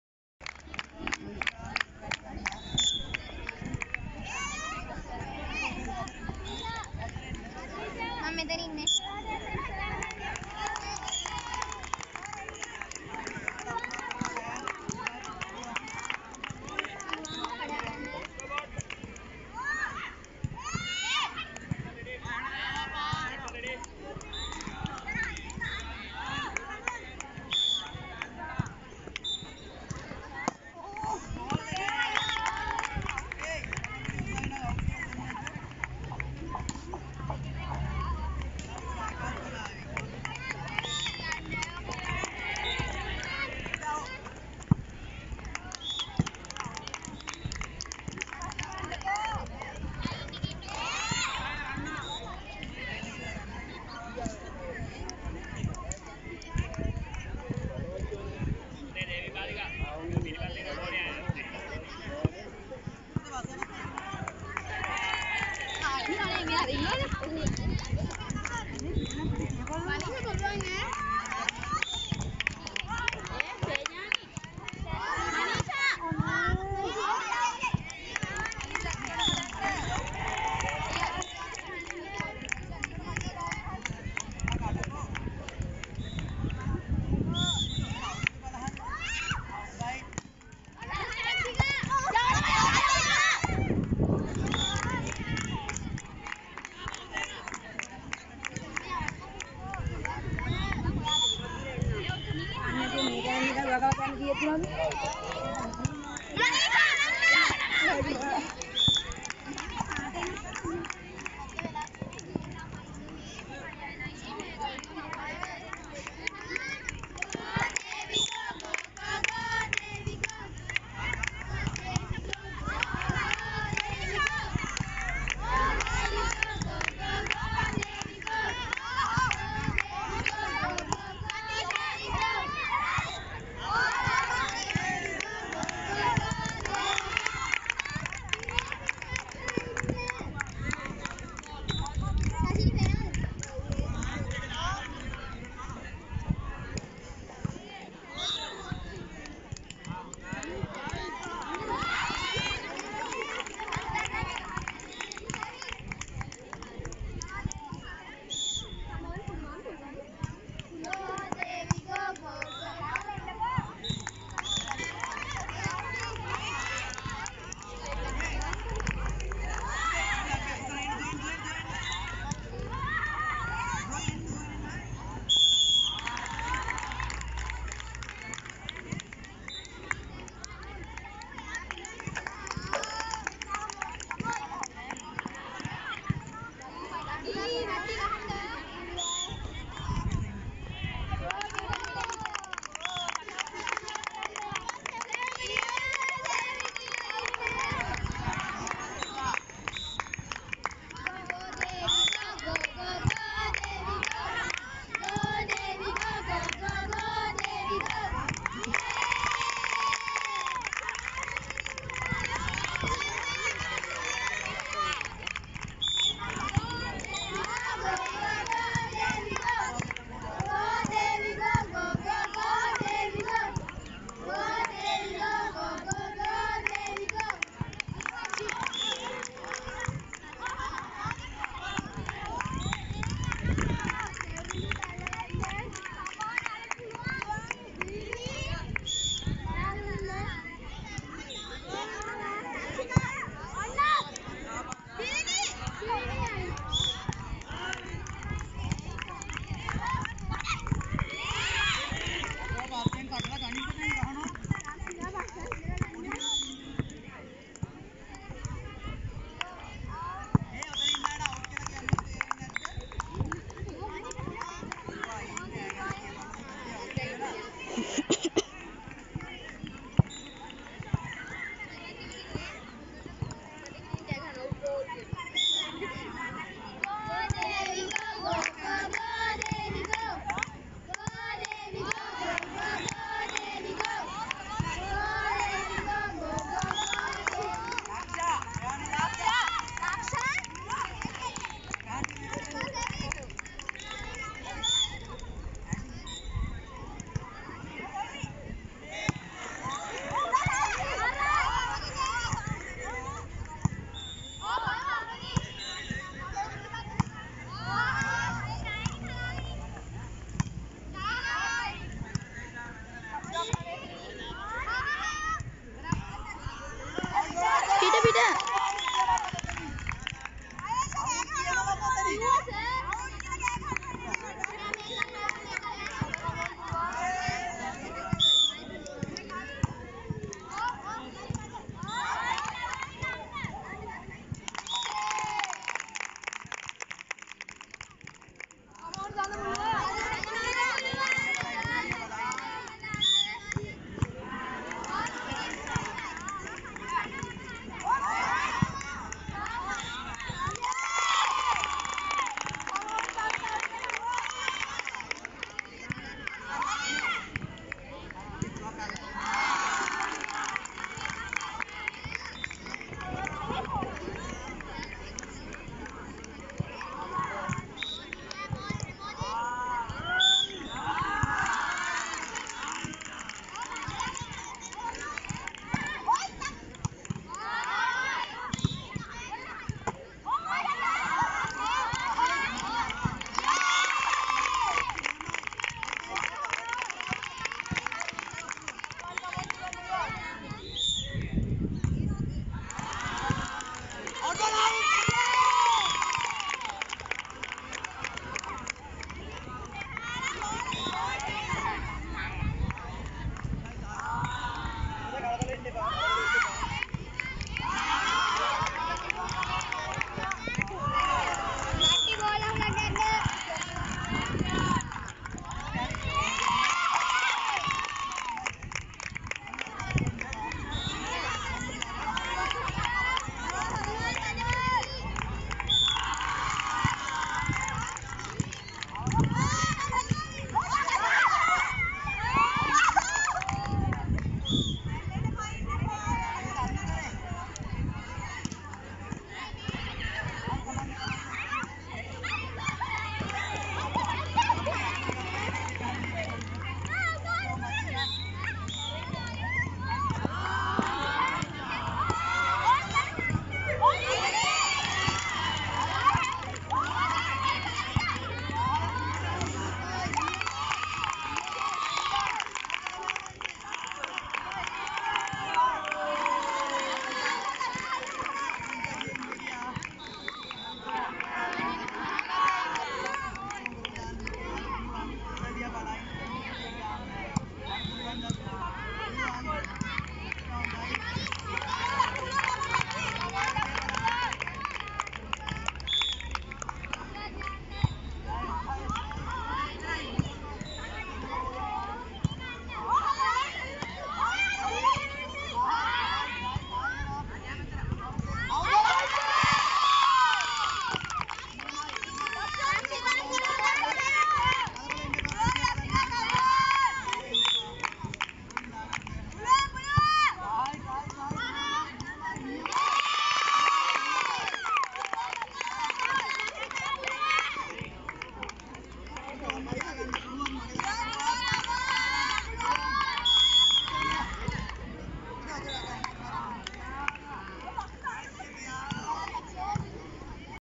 Volleyball Match | Girls | Under 16 | SRI LANKA SCHOOL | Coach RSD
This is the second competition in which these girls will compete.  Their first match is the Mawanella divisional match.  Although an amateur, he shows a moderate amount of talent.  They face the champions or runners-up of the Mawanella Zone almost every year.  Congratulations on their future.
           මෙම දේවි බාලිකාවන් ඉදිරිපත්වන දෙවන තරගය මෙයයි. මොවුන්ගේ පළමු තරගය මාවනැල්ල කොට්ඨාශ තරගයි.  ආධුනික වුවද මධ්‍යස්ථ ප්‍රමාණයේ දක්ශතා පෙන්වනු ලබයි. ඔවුන් මුහුණ දෙනු ලබන්නේ සෑම වසරකම පාහේ මාවනැල්ල කලාපයේ ශූරතාවය හෝ අනුශූරතාවය ලබා ගන්නා කණ්ඩායම සමගයි. ඔවුන්ගේ අනාගතයට සුභ පතමු.

Schoolgirls volleyball match in Mawanella Educational Zonal - part 1
Kg / Mw / Devi Balika Vidyalaya - 2020